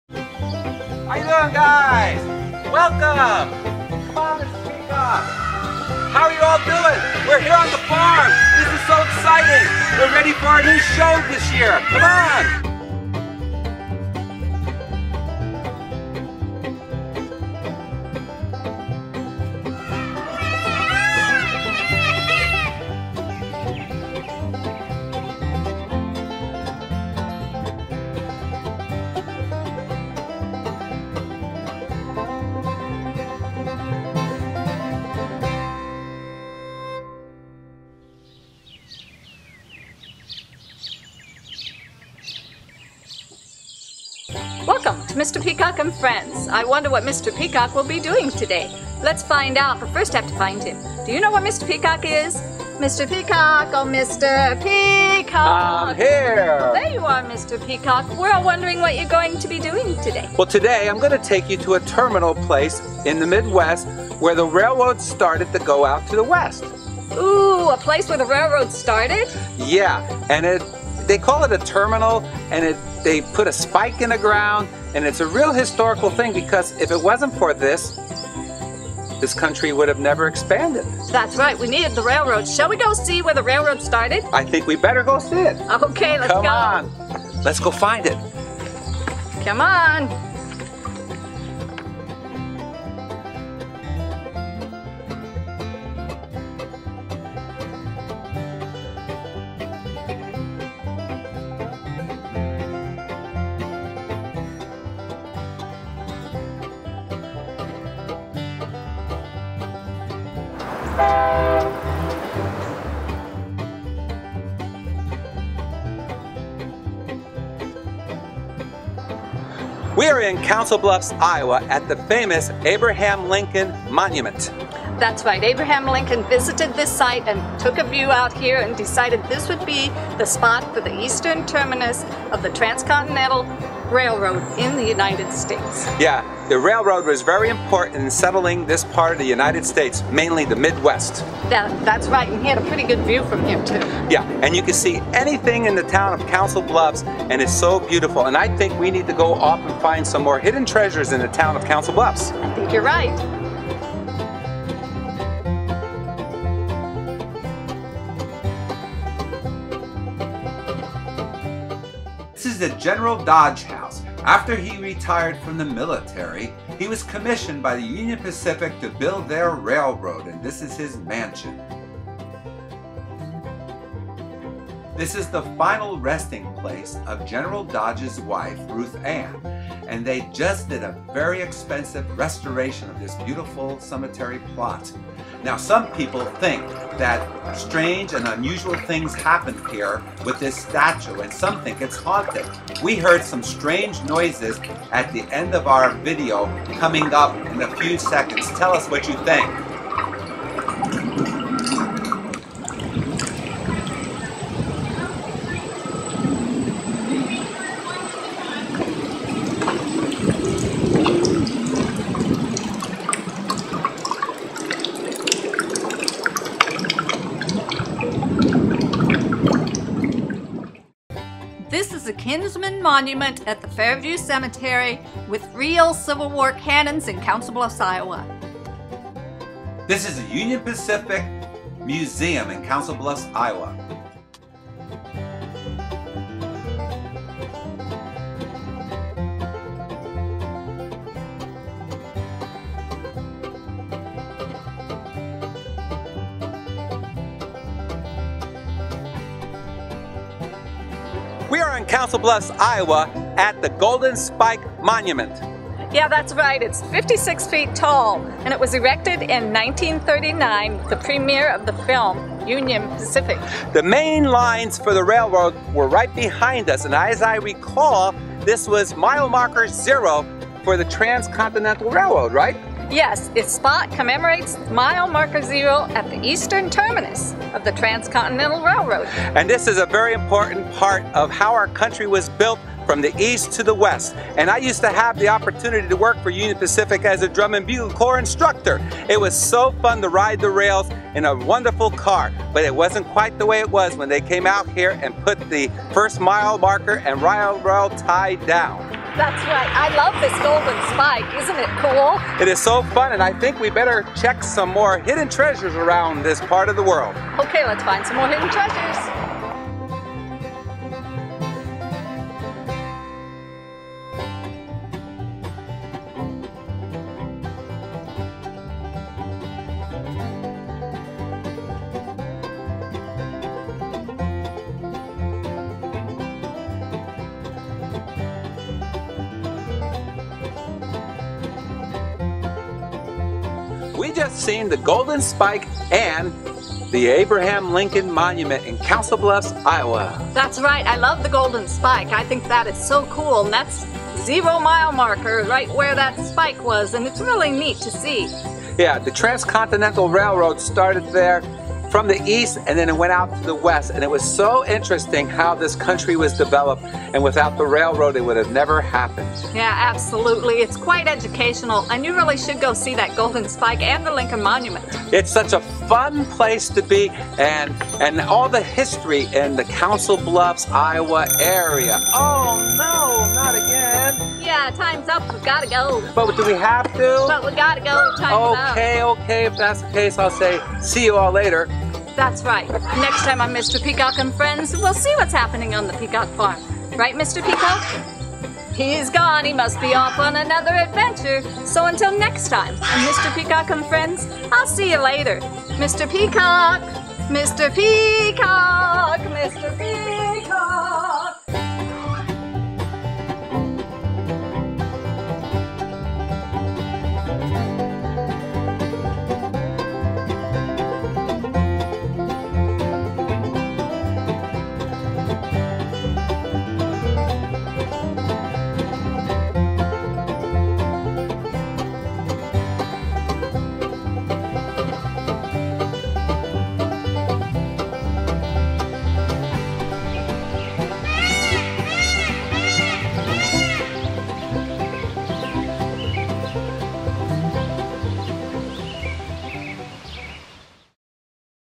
Golden Spike Lincoln Monument, Council Bluffs, Ia Travel USA, Mr Peacock & Friends Hidden Treasures
On Mr. Peacock's latest adventure, they discovered a hidden treasure of the Golden Spike, Lincoln Monument and the Black Angel in Council Bluffs, Iowa.   The town of Council Bluffs, Iowa is steeped in historical memories of the past. With the founding of the eastern terminus for the Union Pacific Railroad, You will see the General Dodge house/museum  along with the Black Angel monument that is a tribute to the late Ruth Ann Dodge, wife of General Dodge.   The black Angel has a lot of supernatural folklore associated with it. And during the filming we didn't notice anything strange but when we were editing this video we heard some moaning in the background.  We think it was the wind, but is it? We never had anything like this happen with the hundreds of hours of video that we shot.

Please join us for every episode of Mr. Peacock & Friends season 3 by visiting Mr. Peacock & Friends web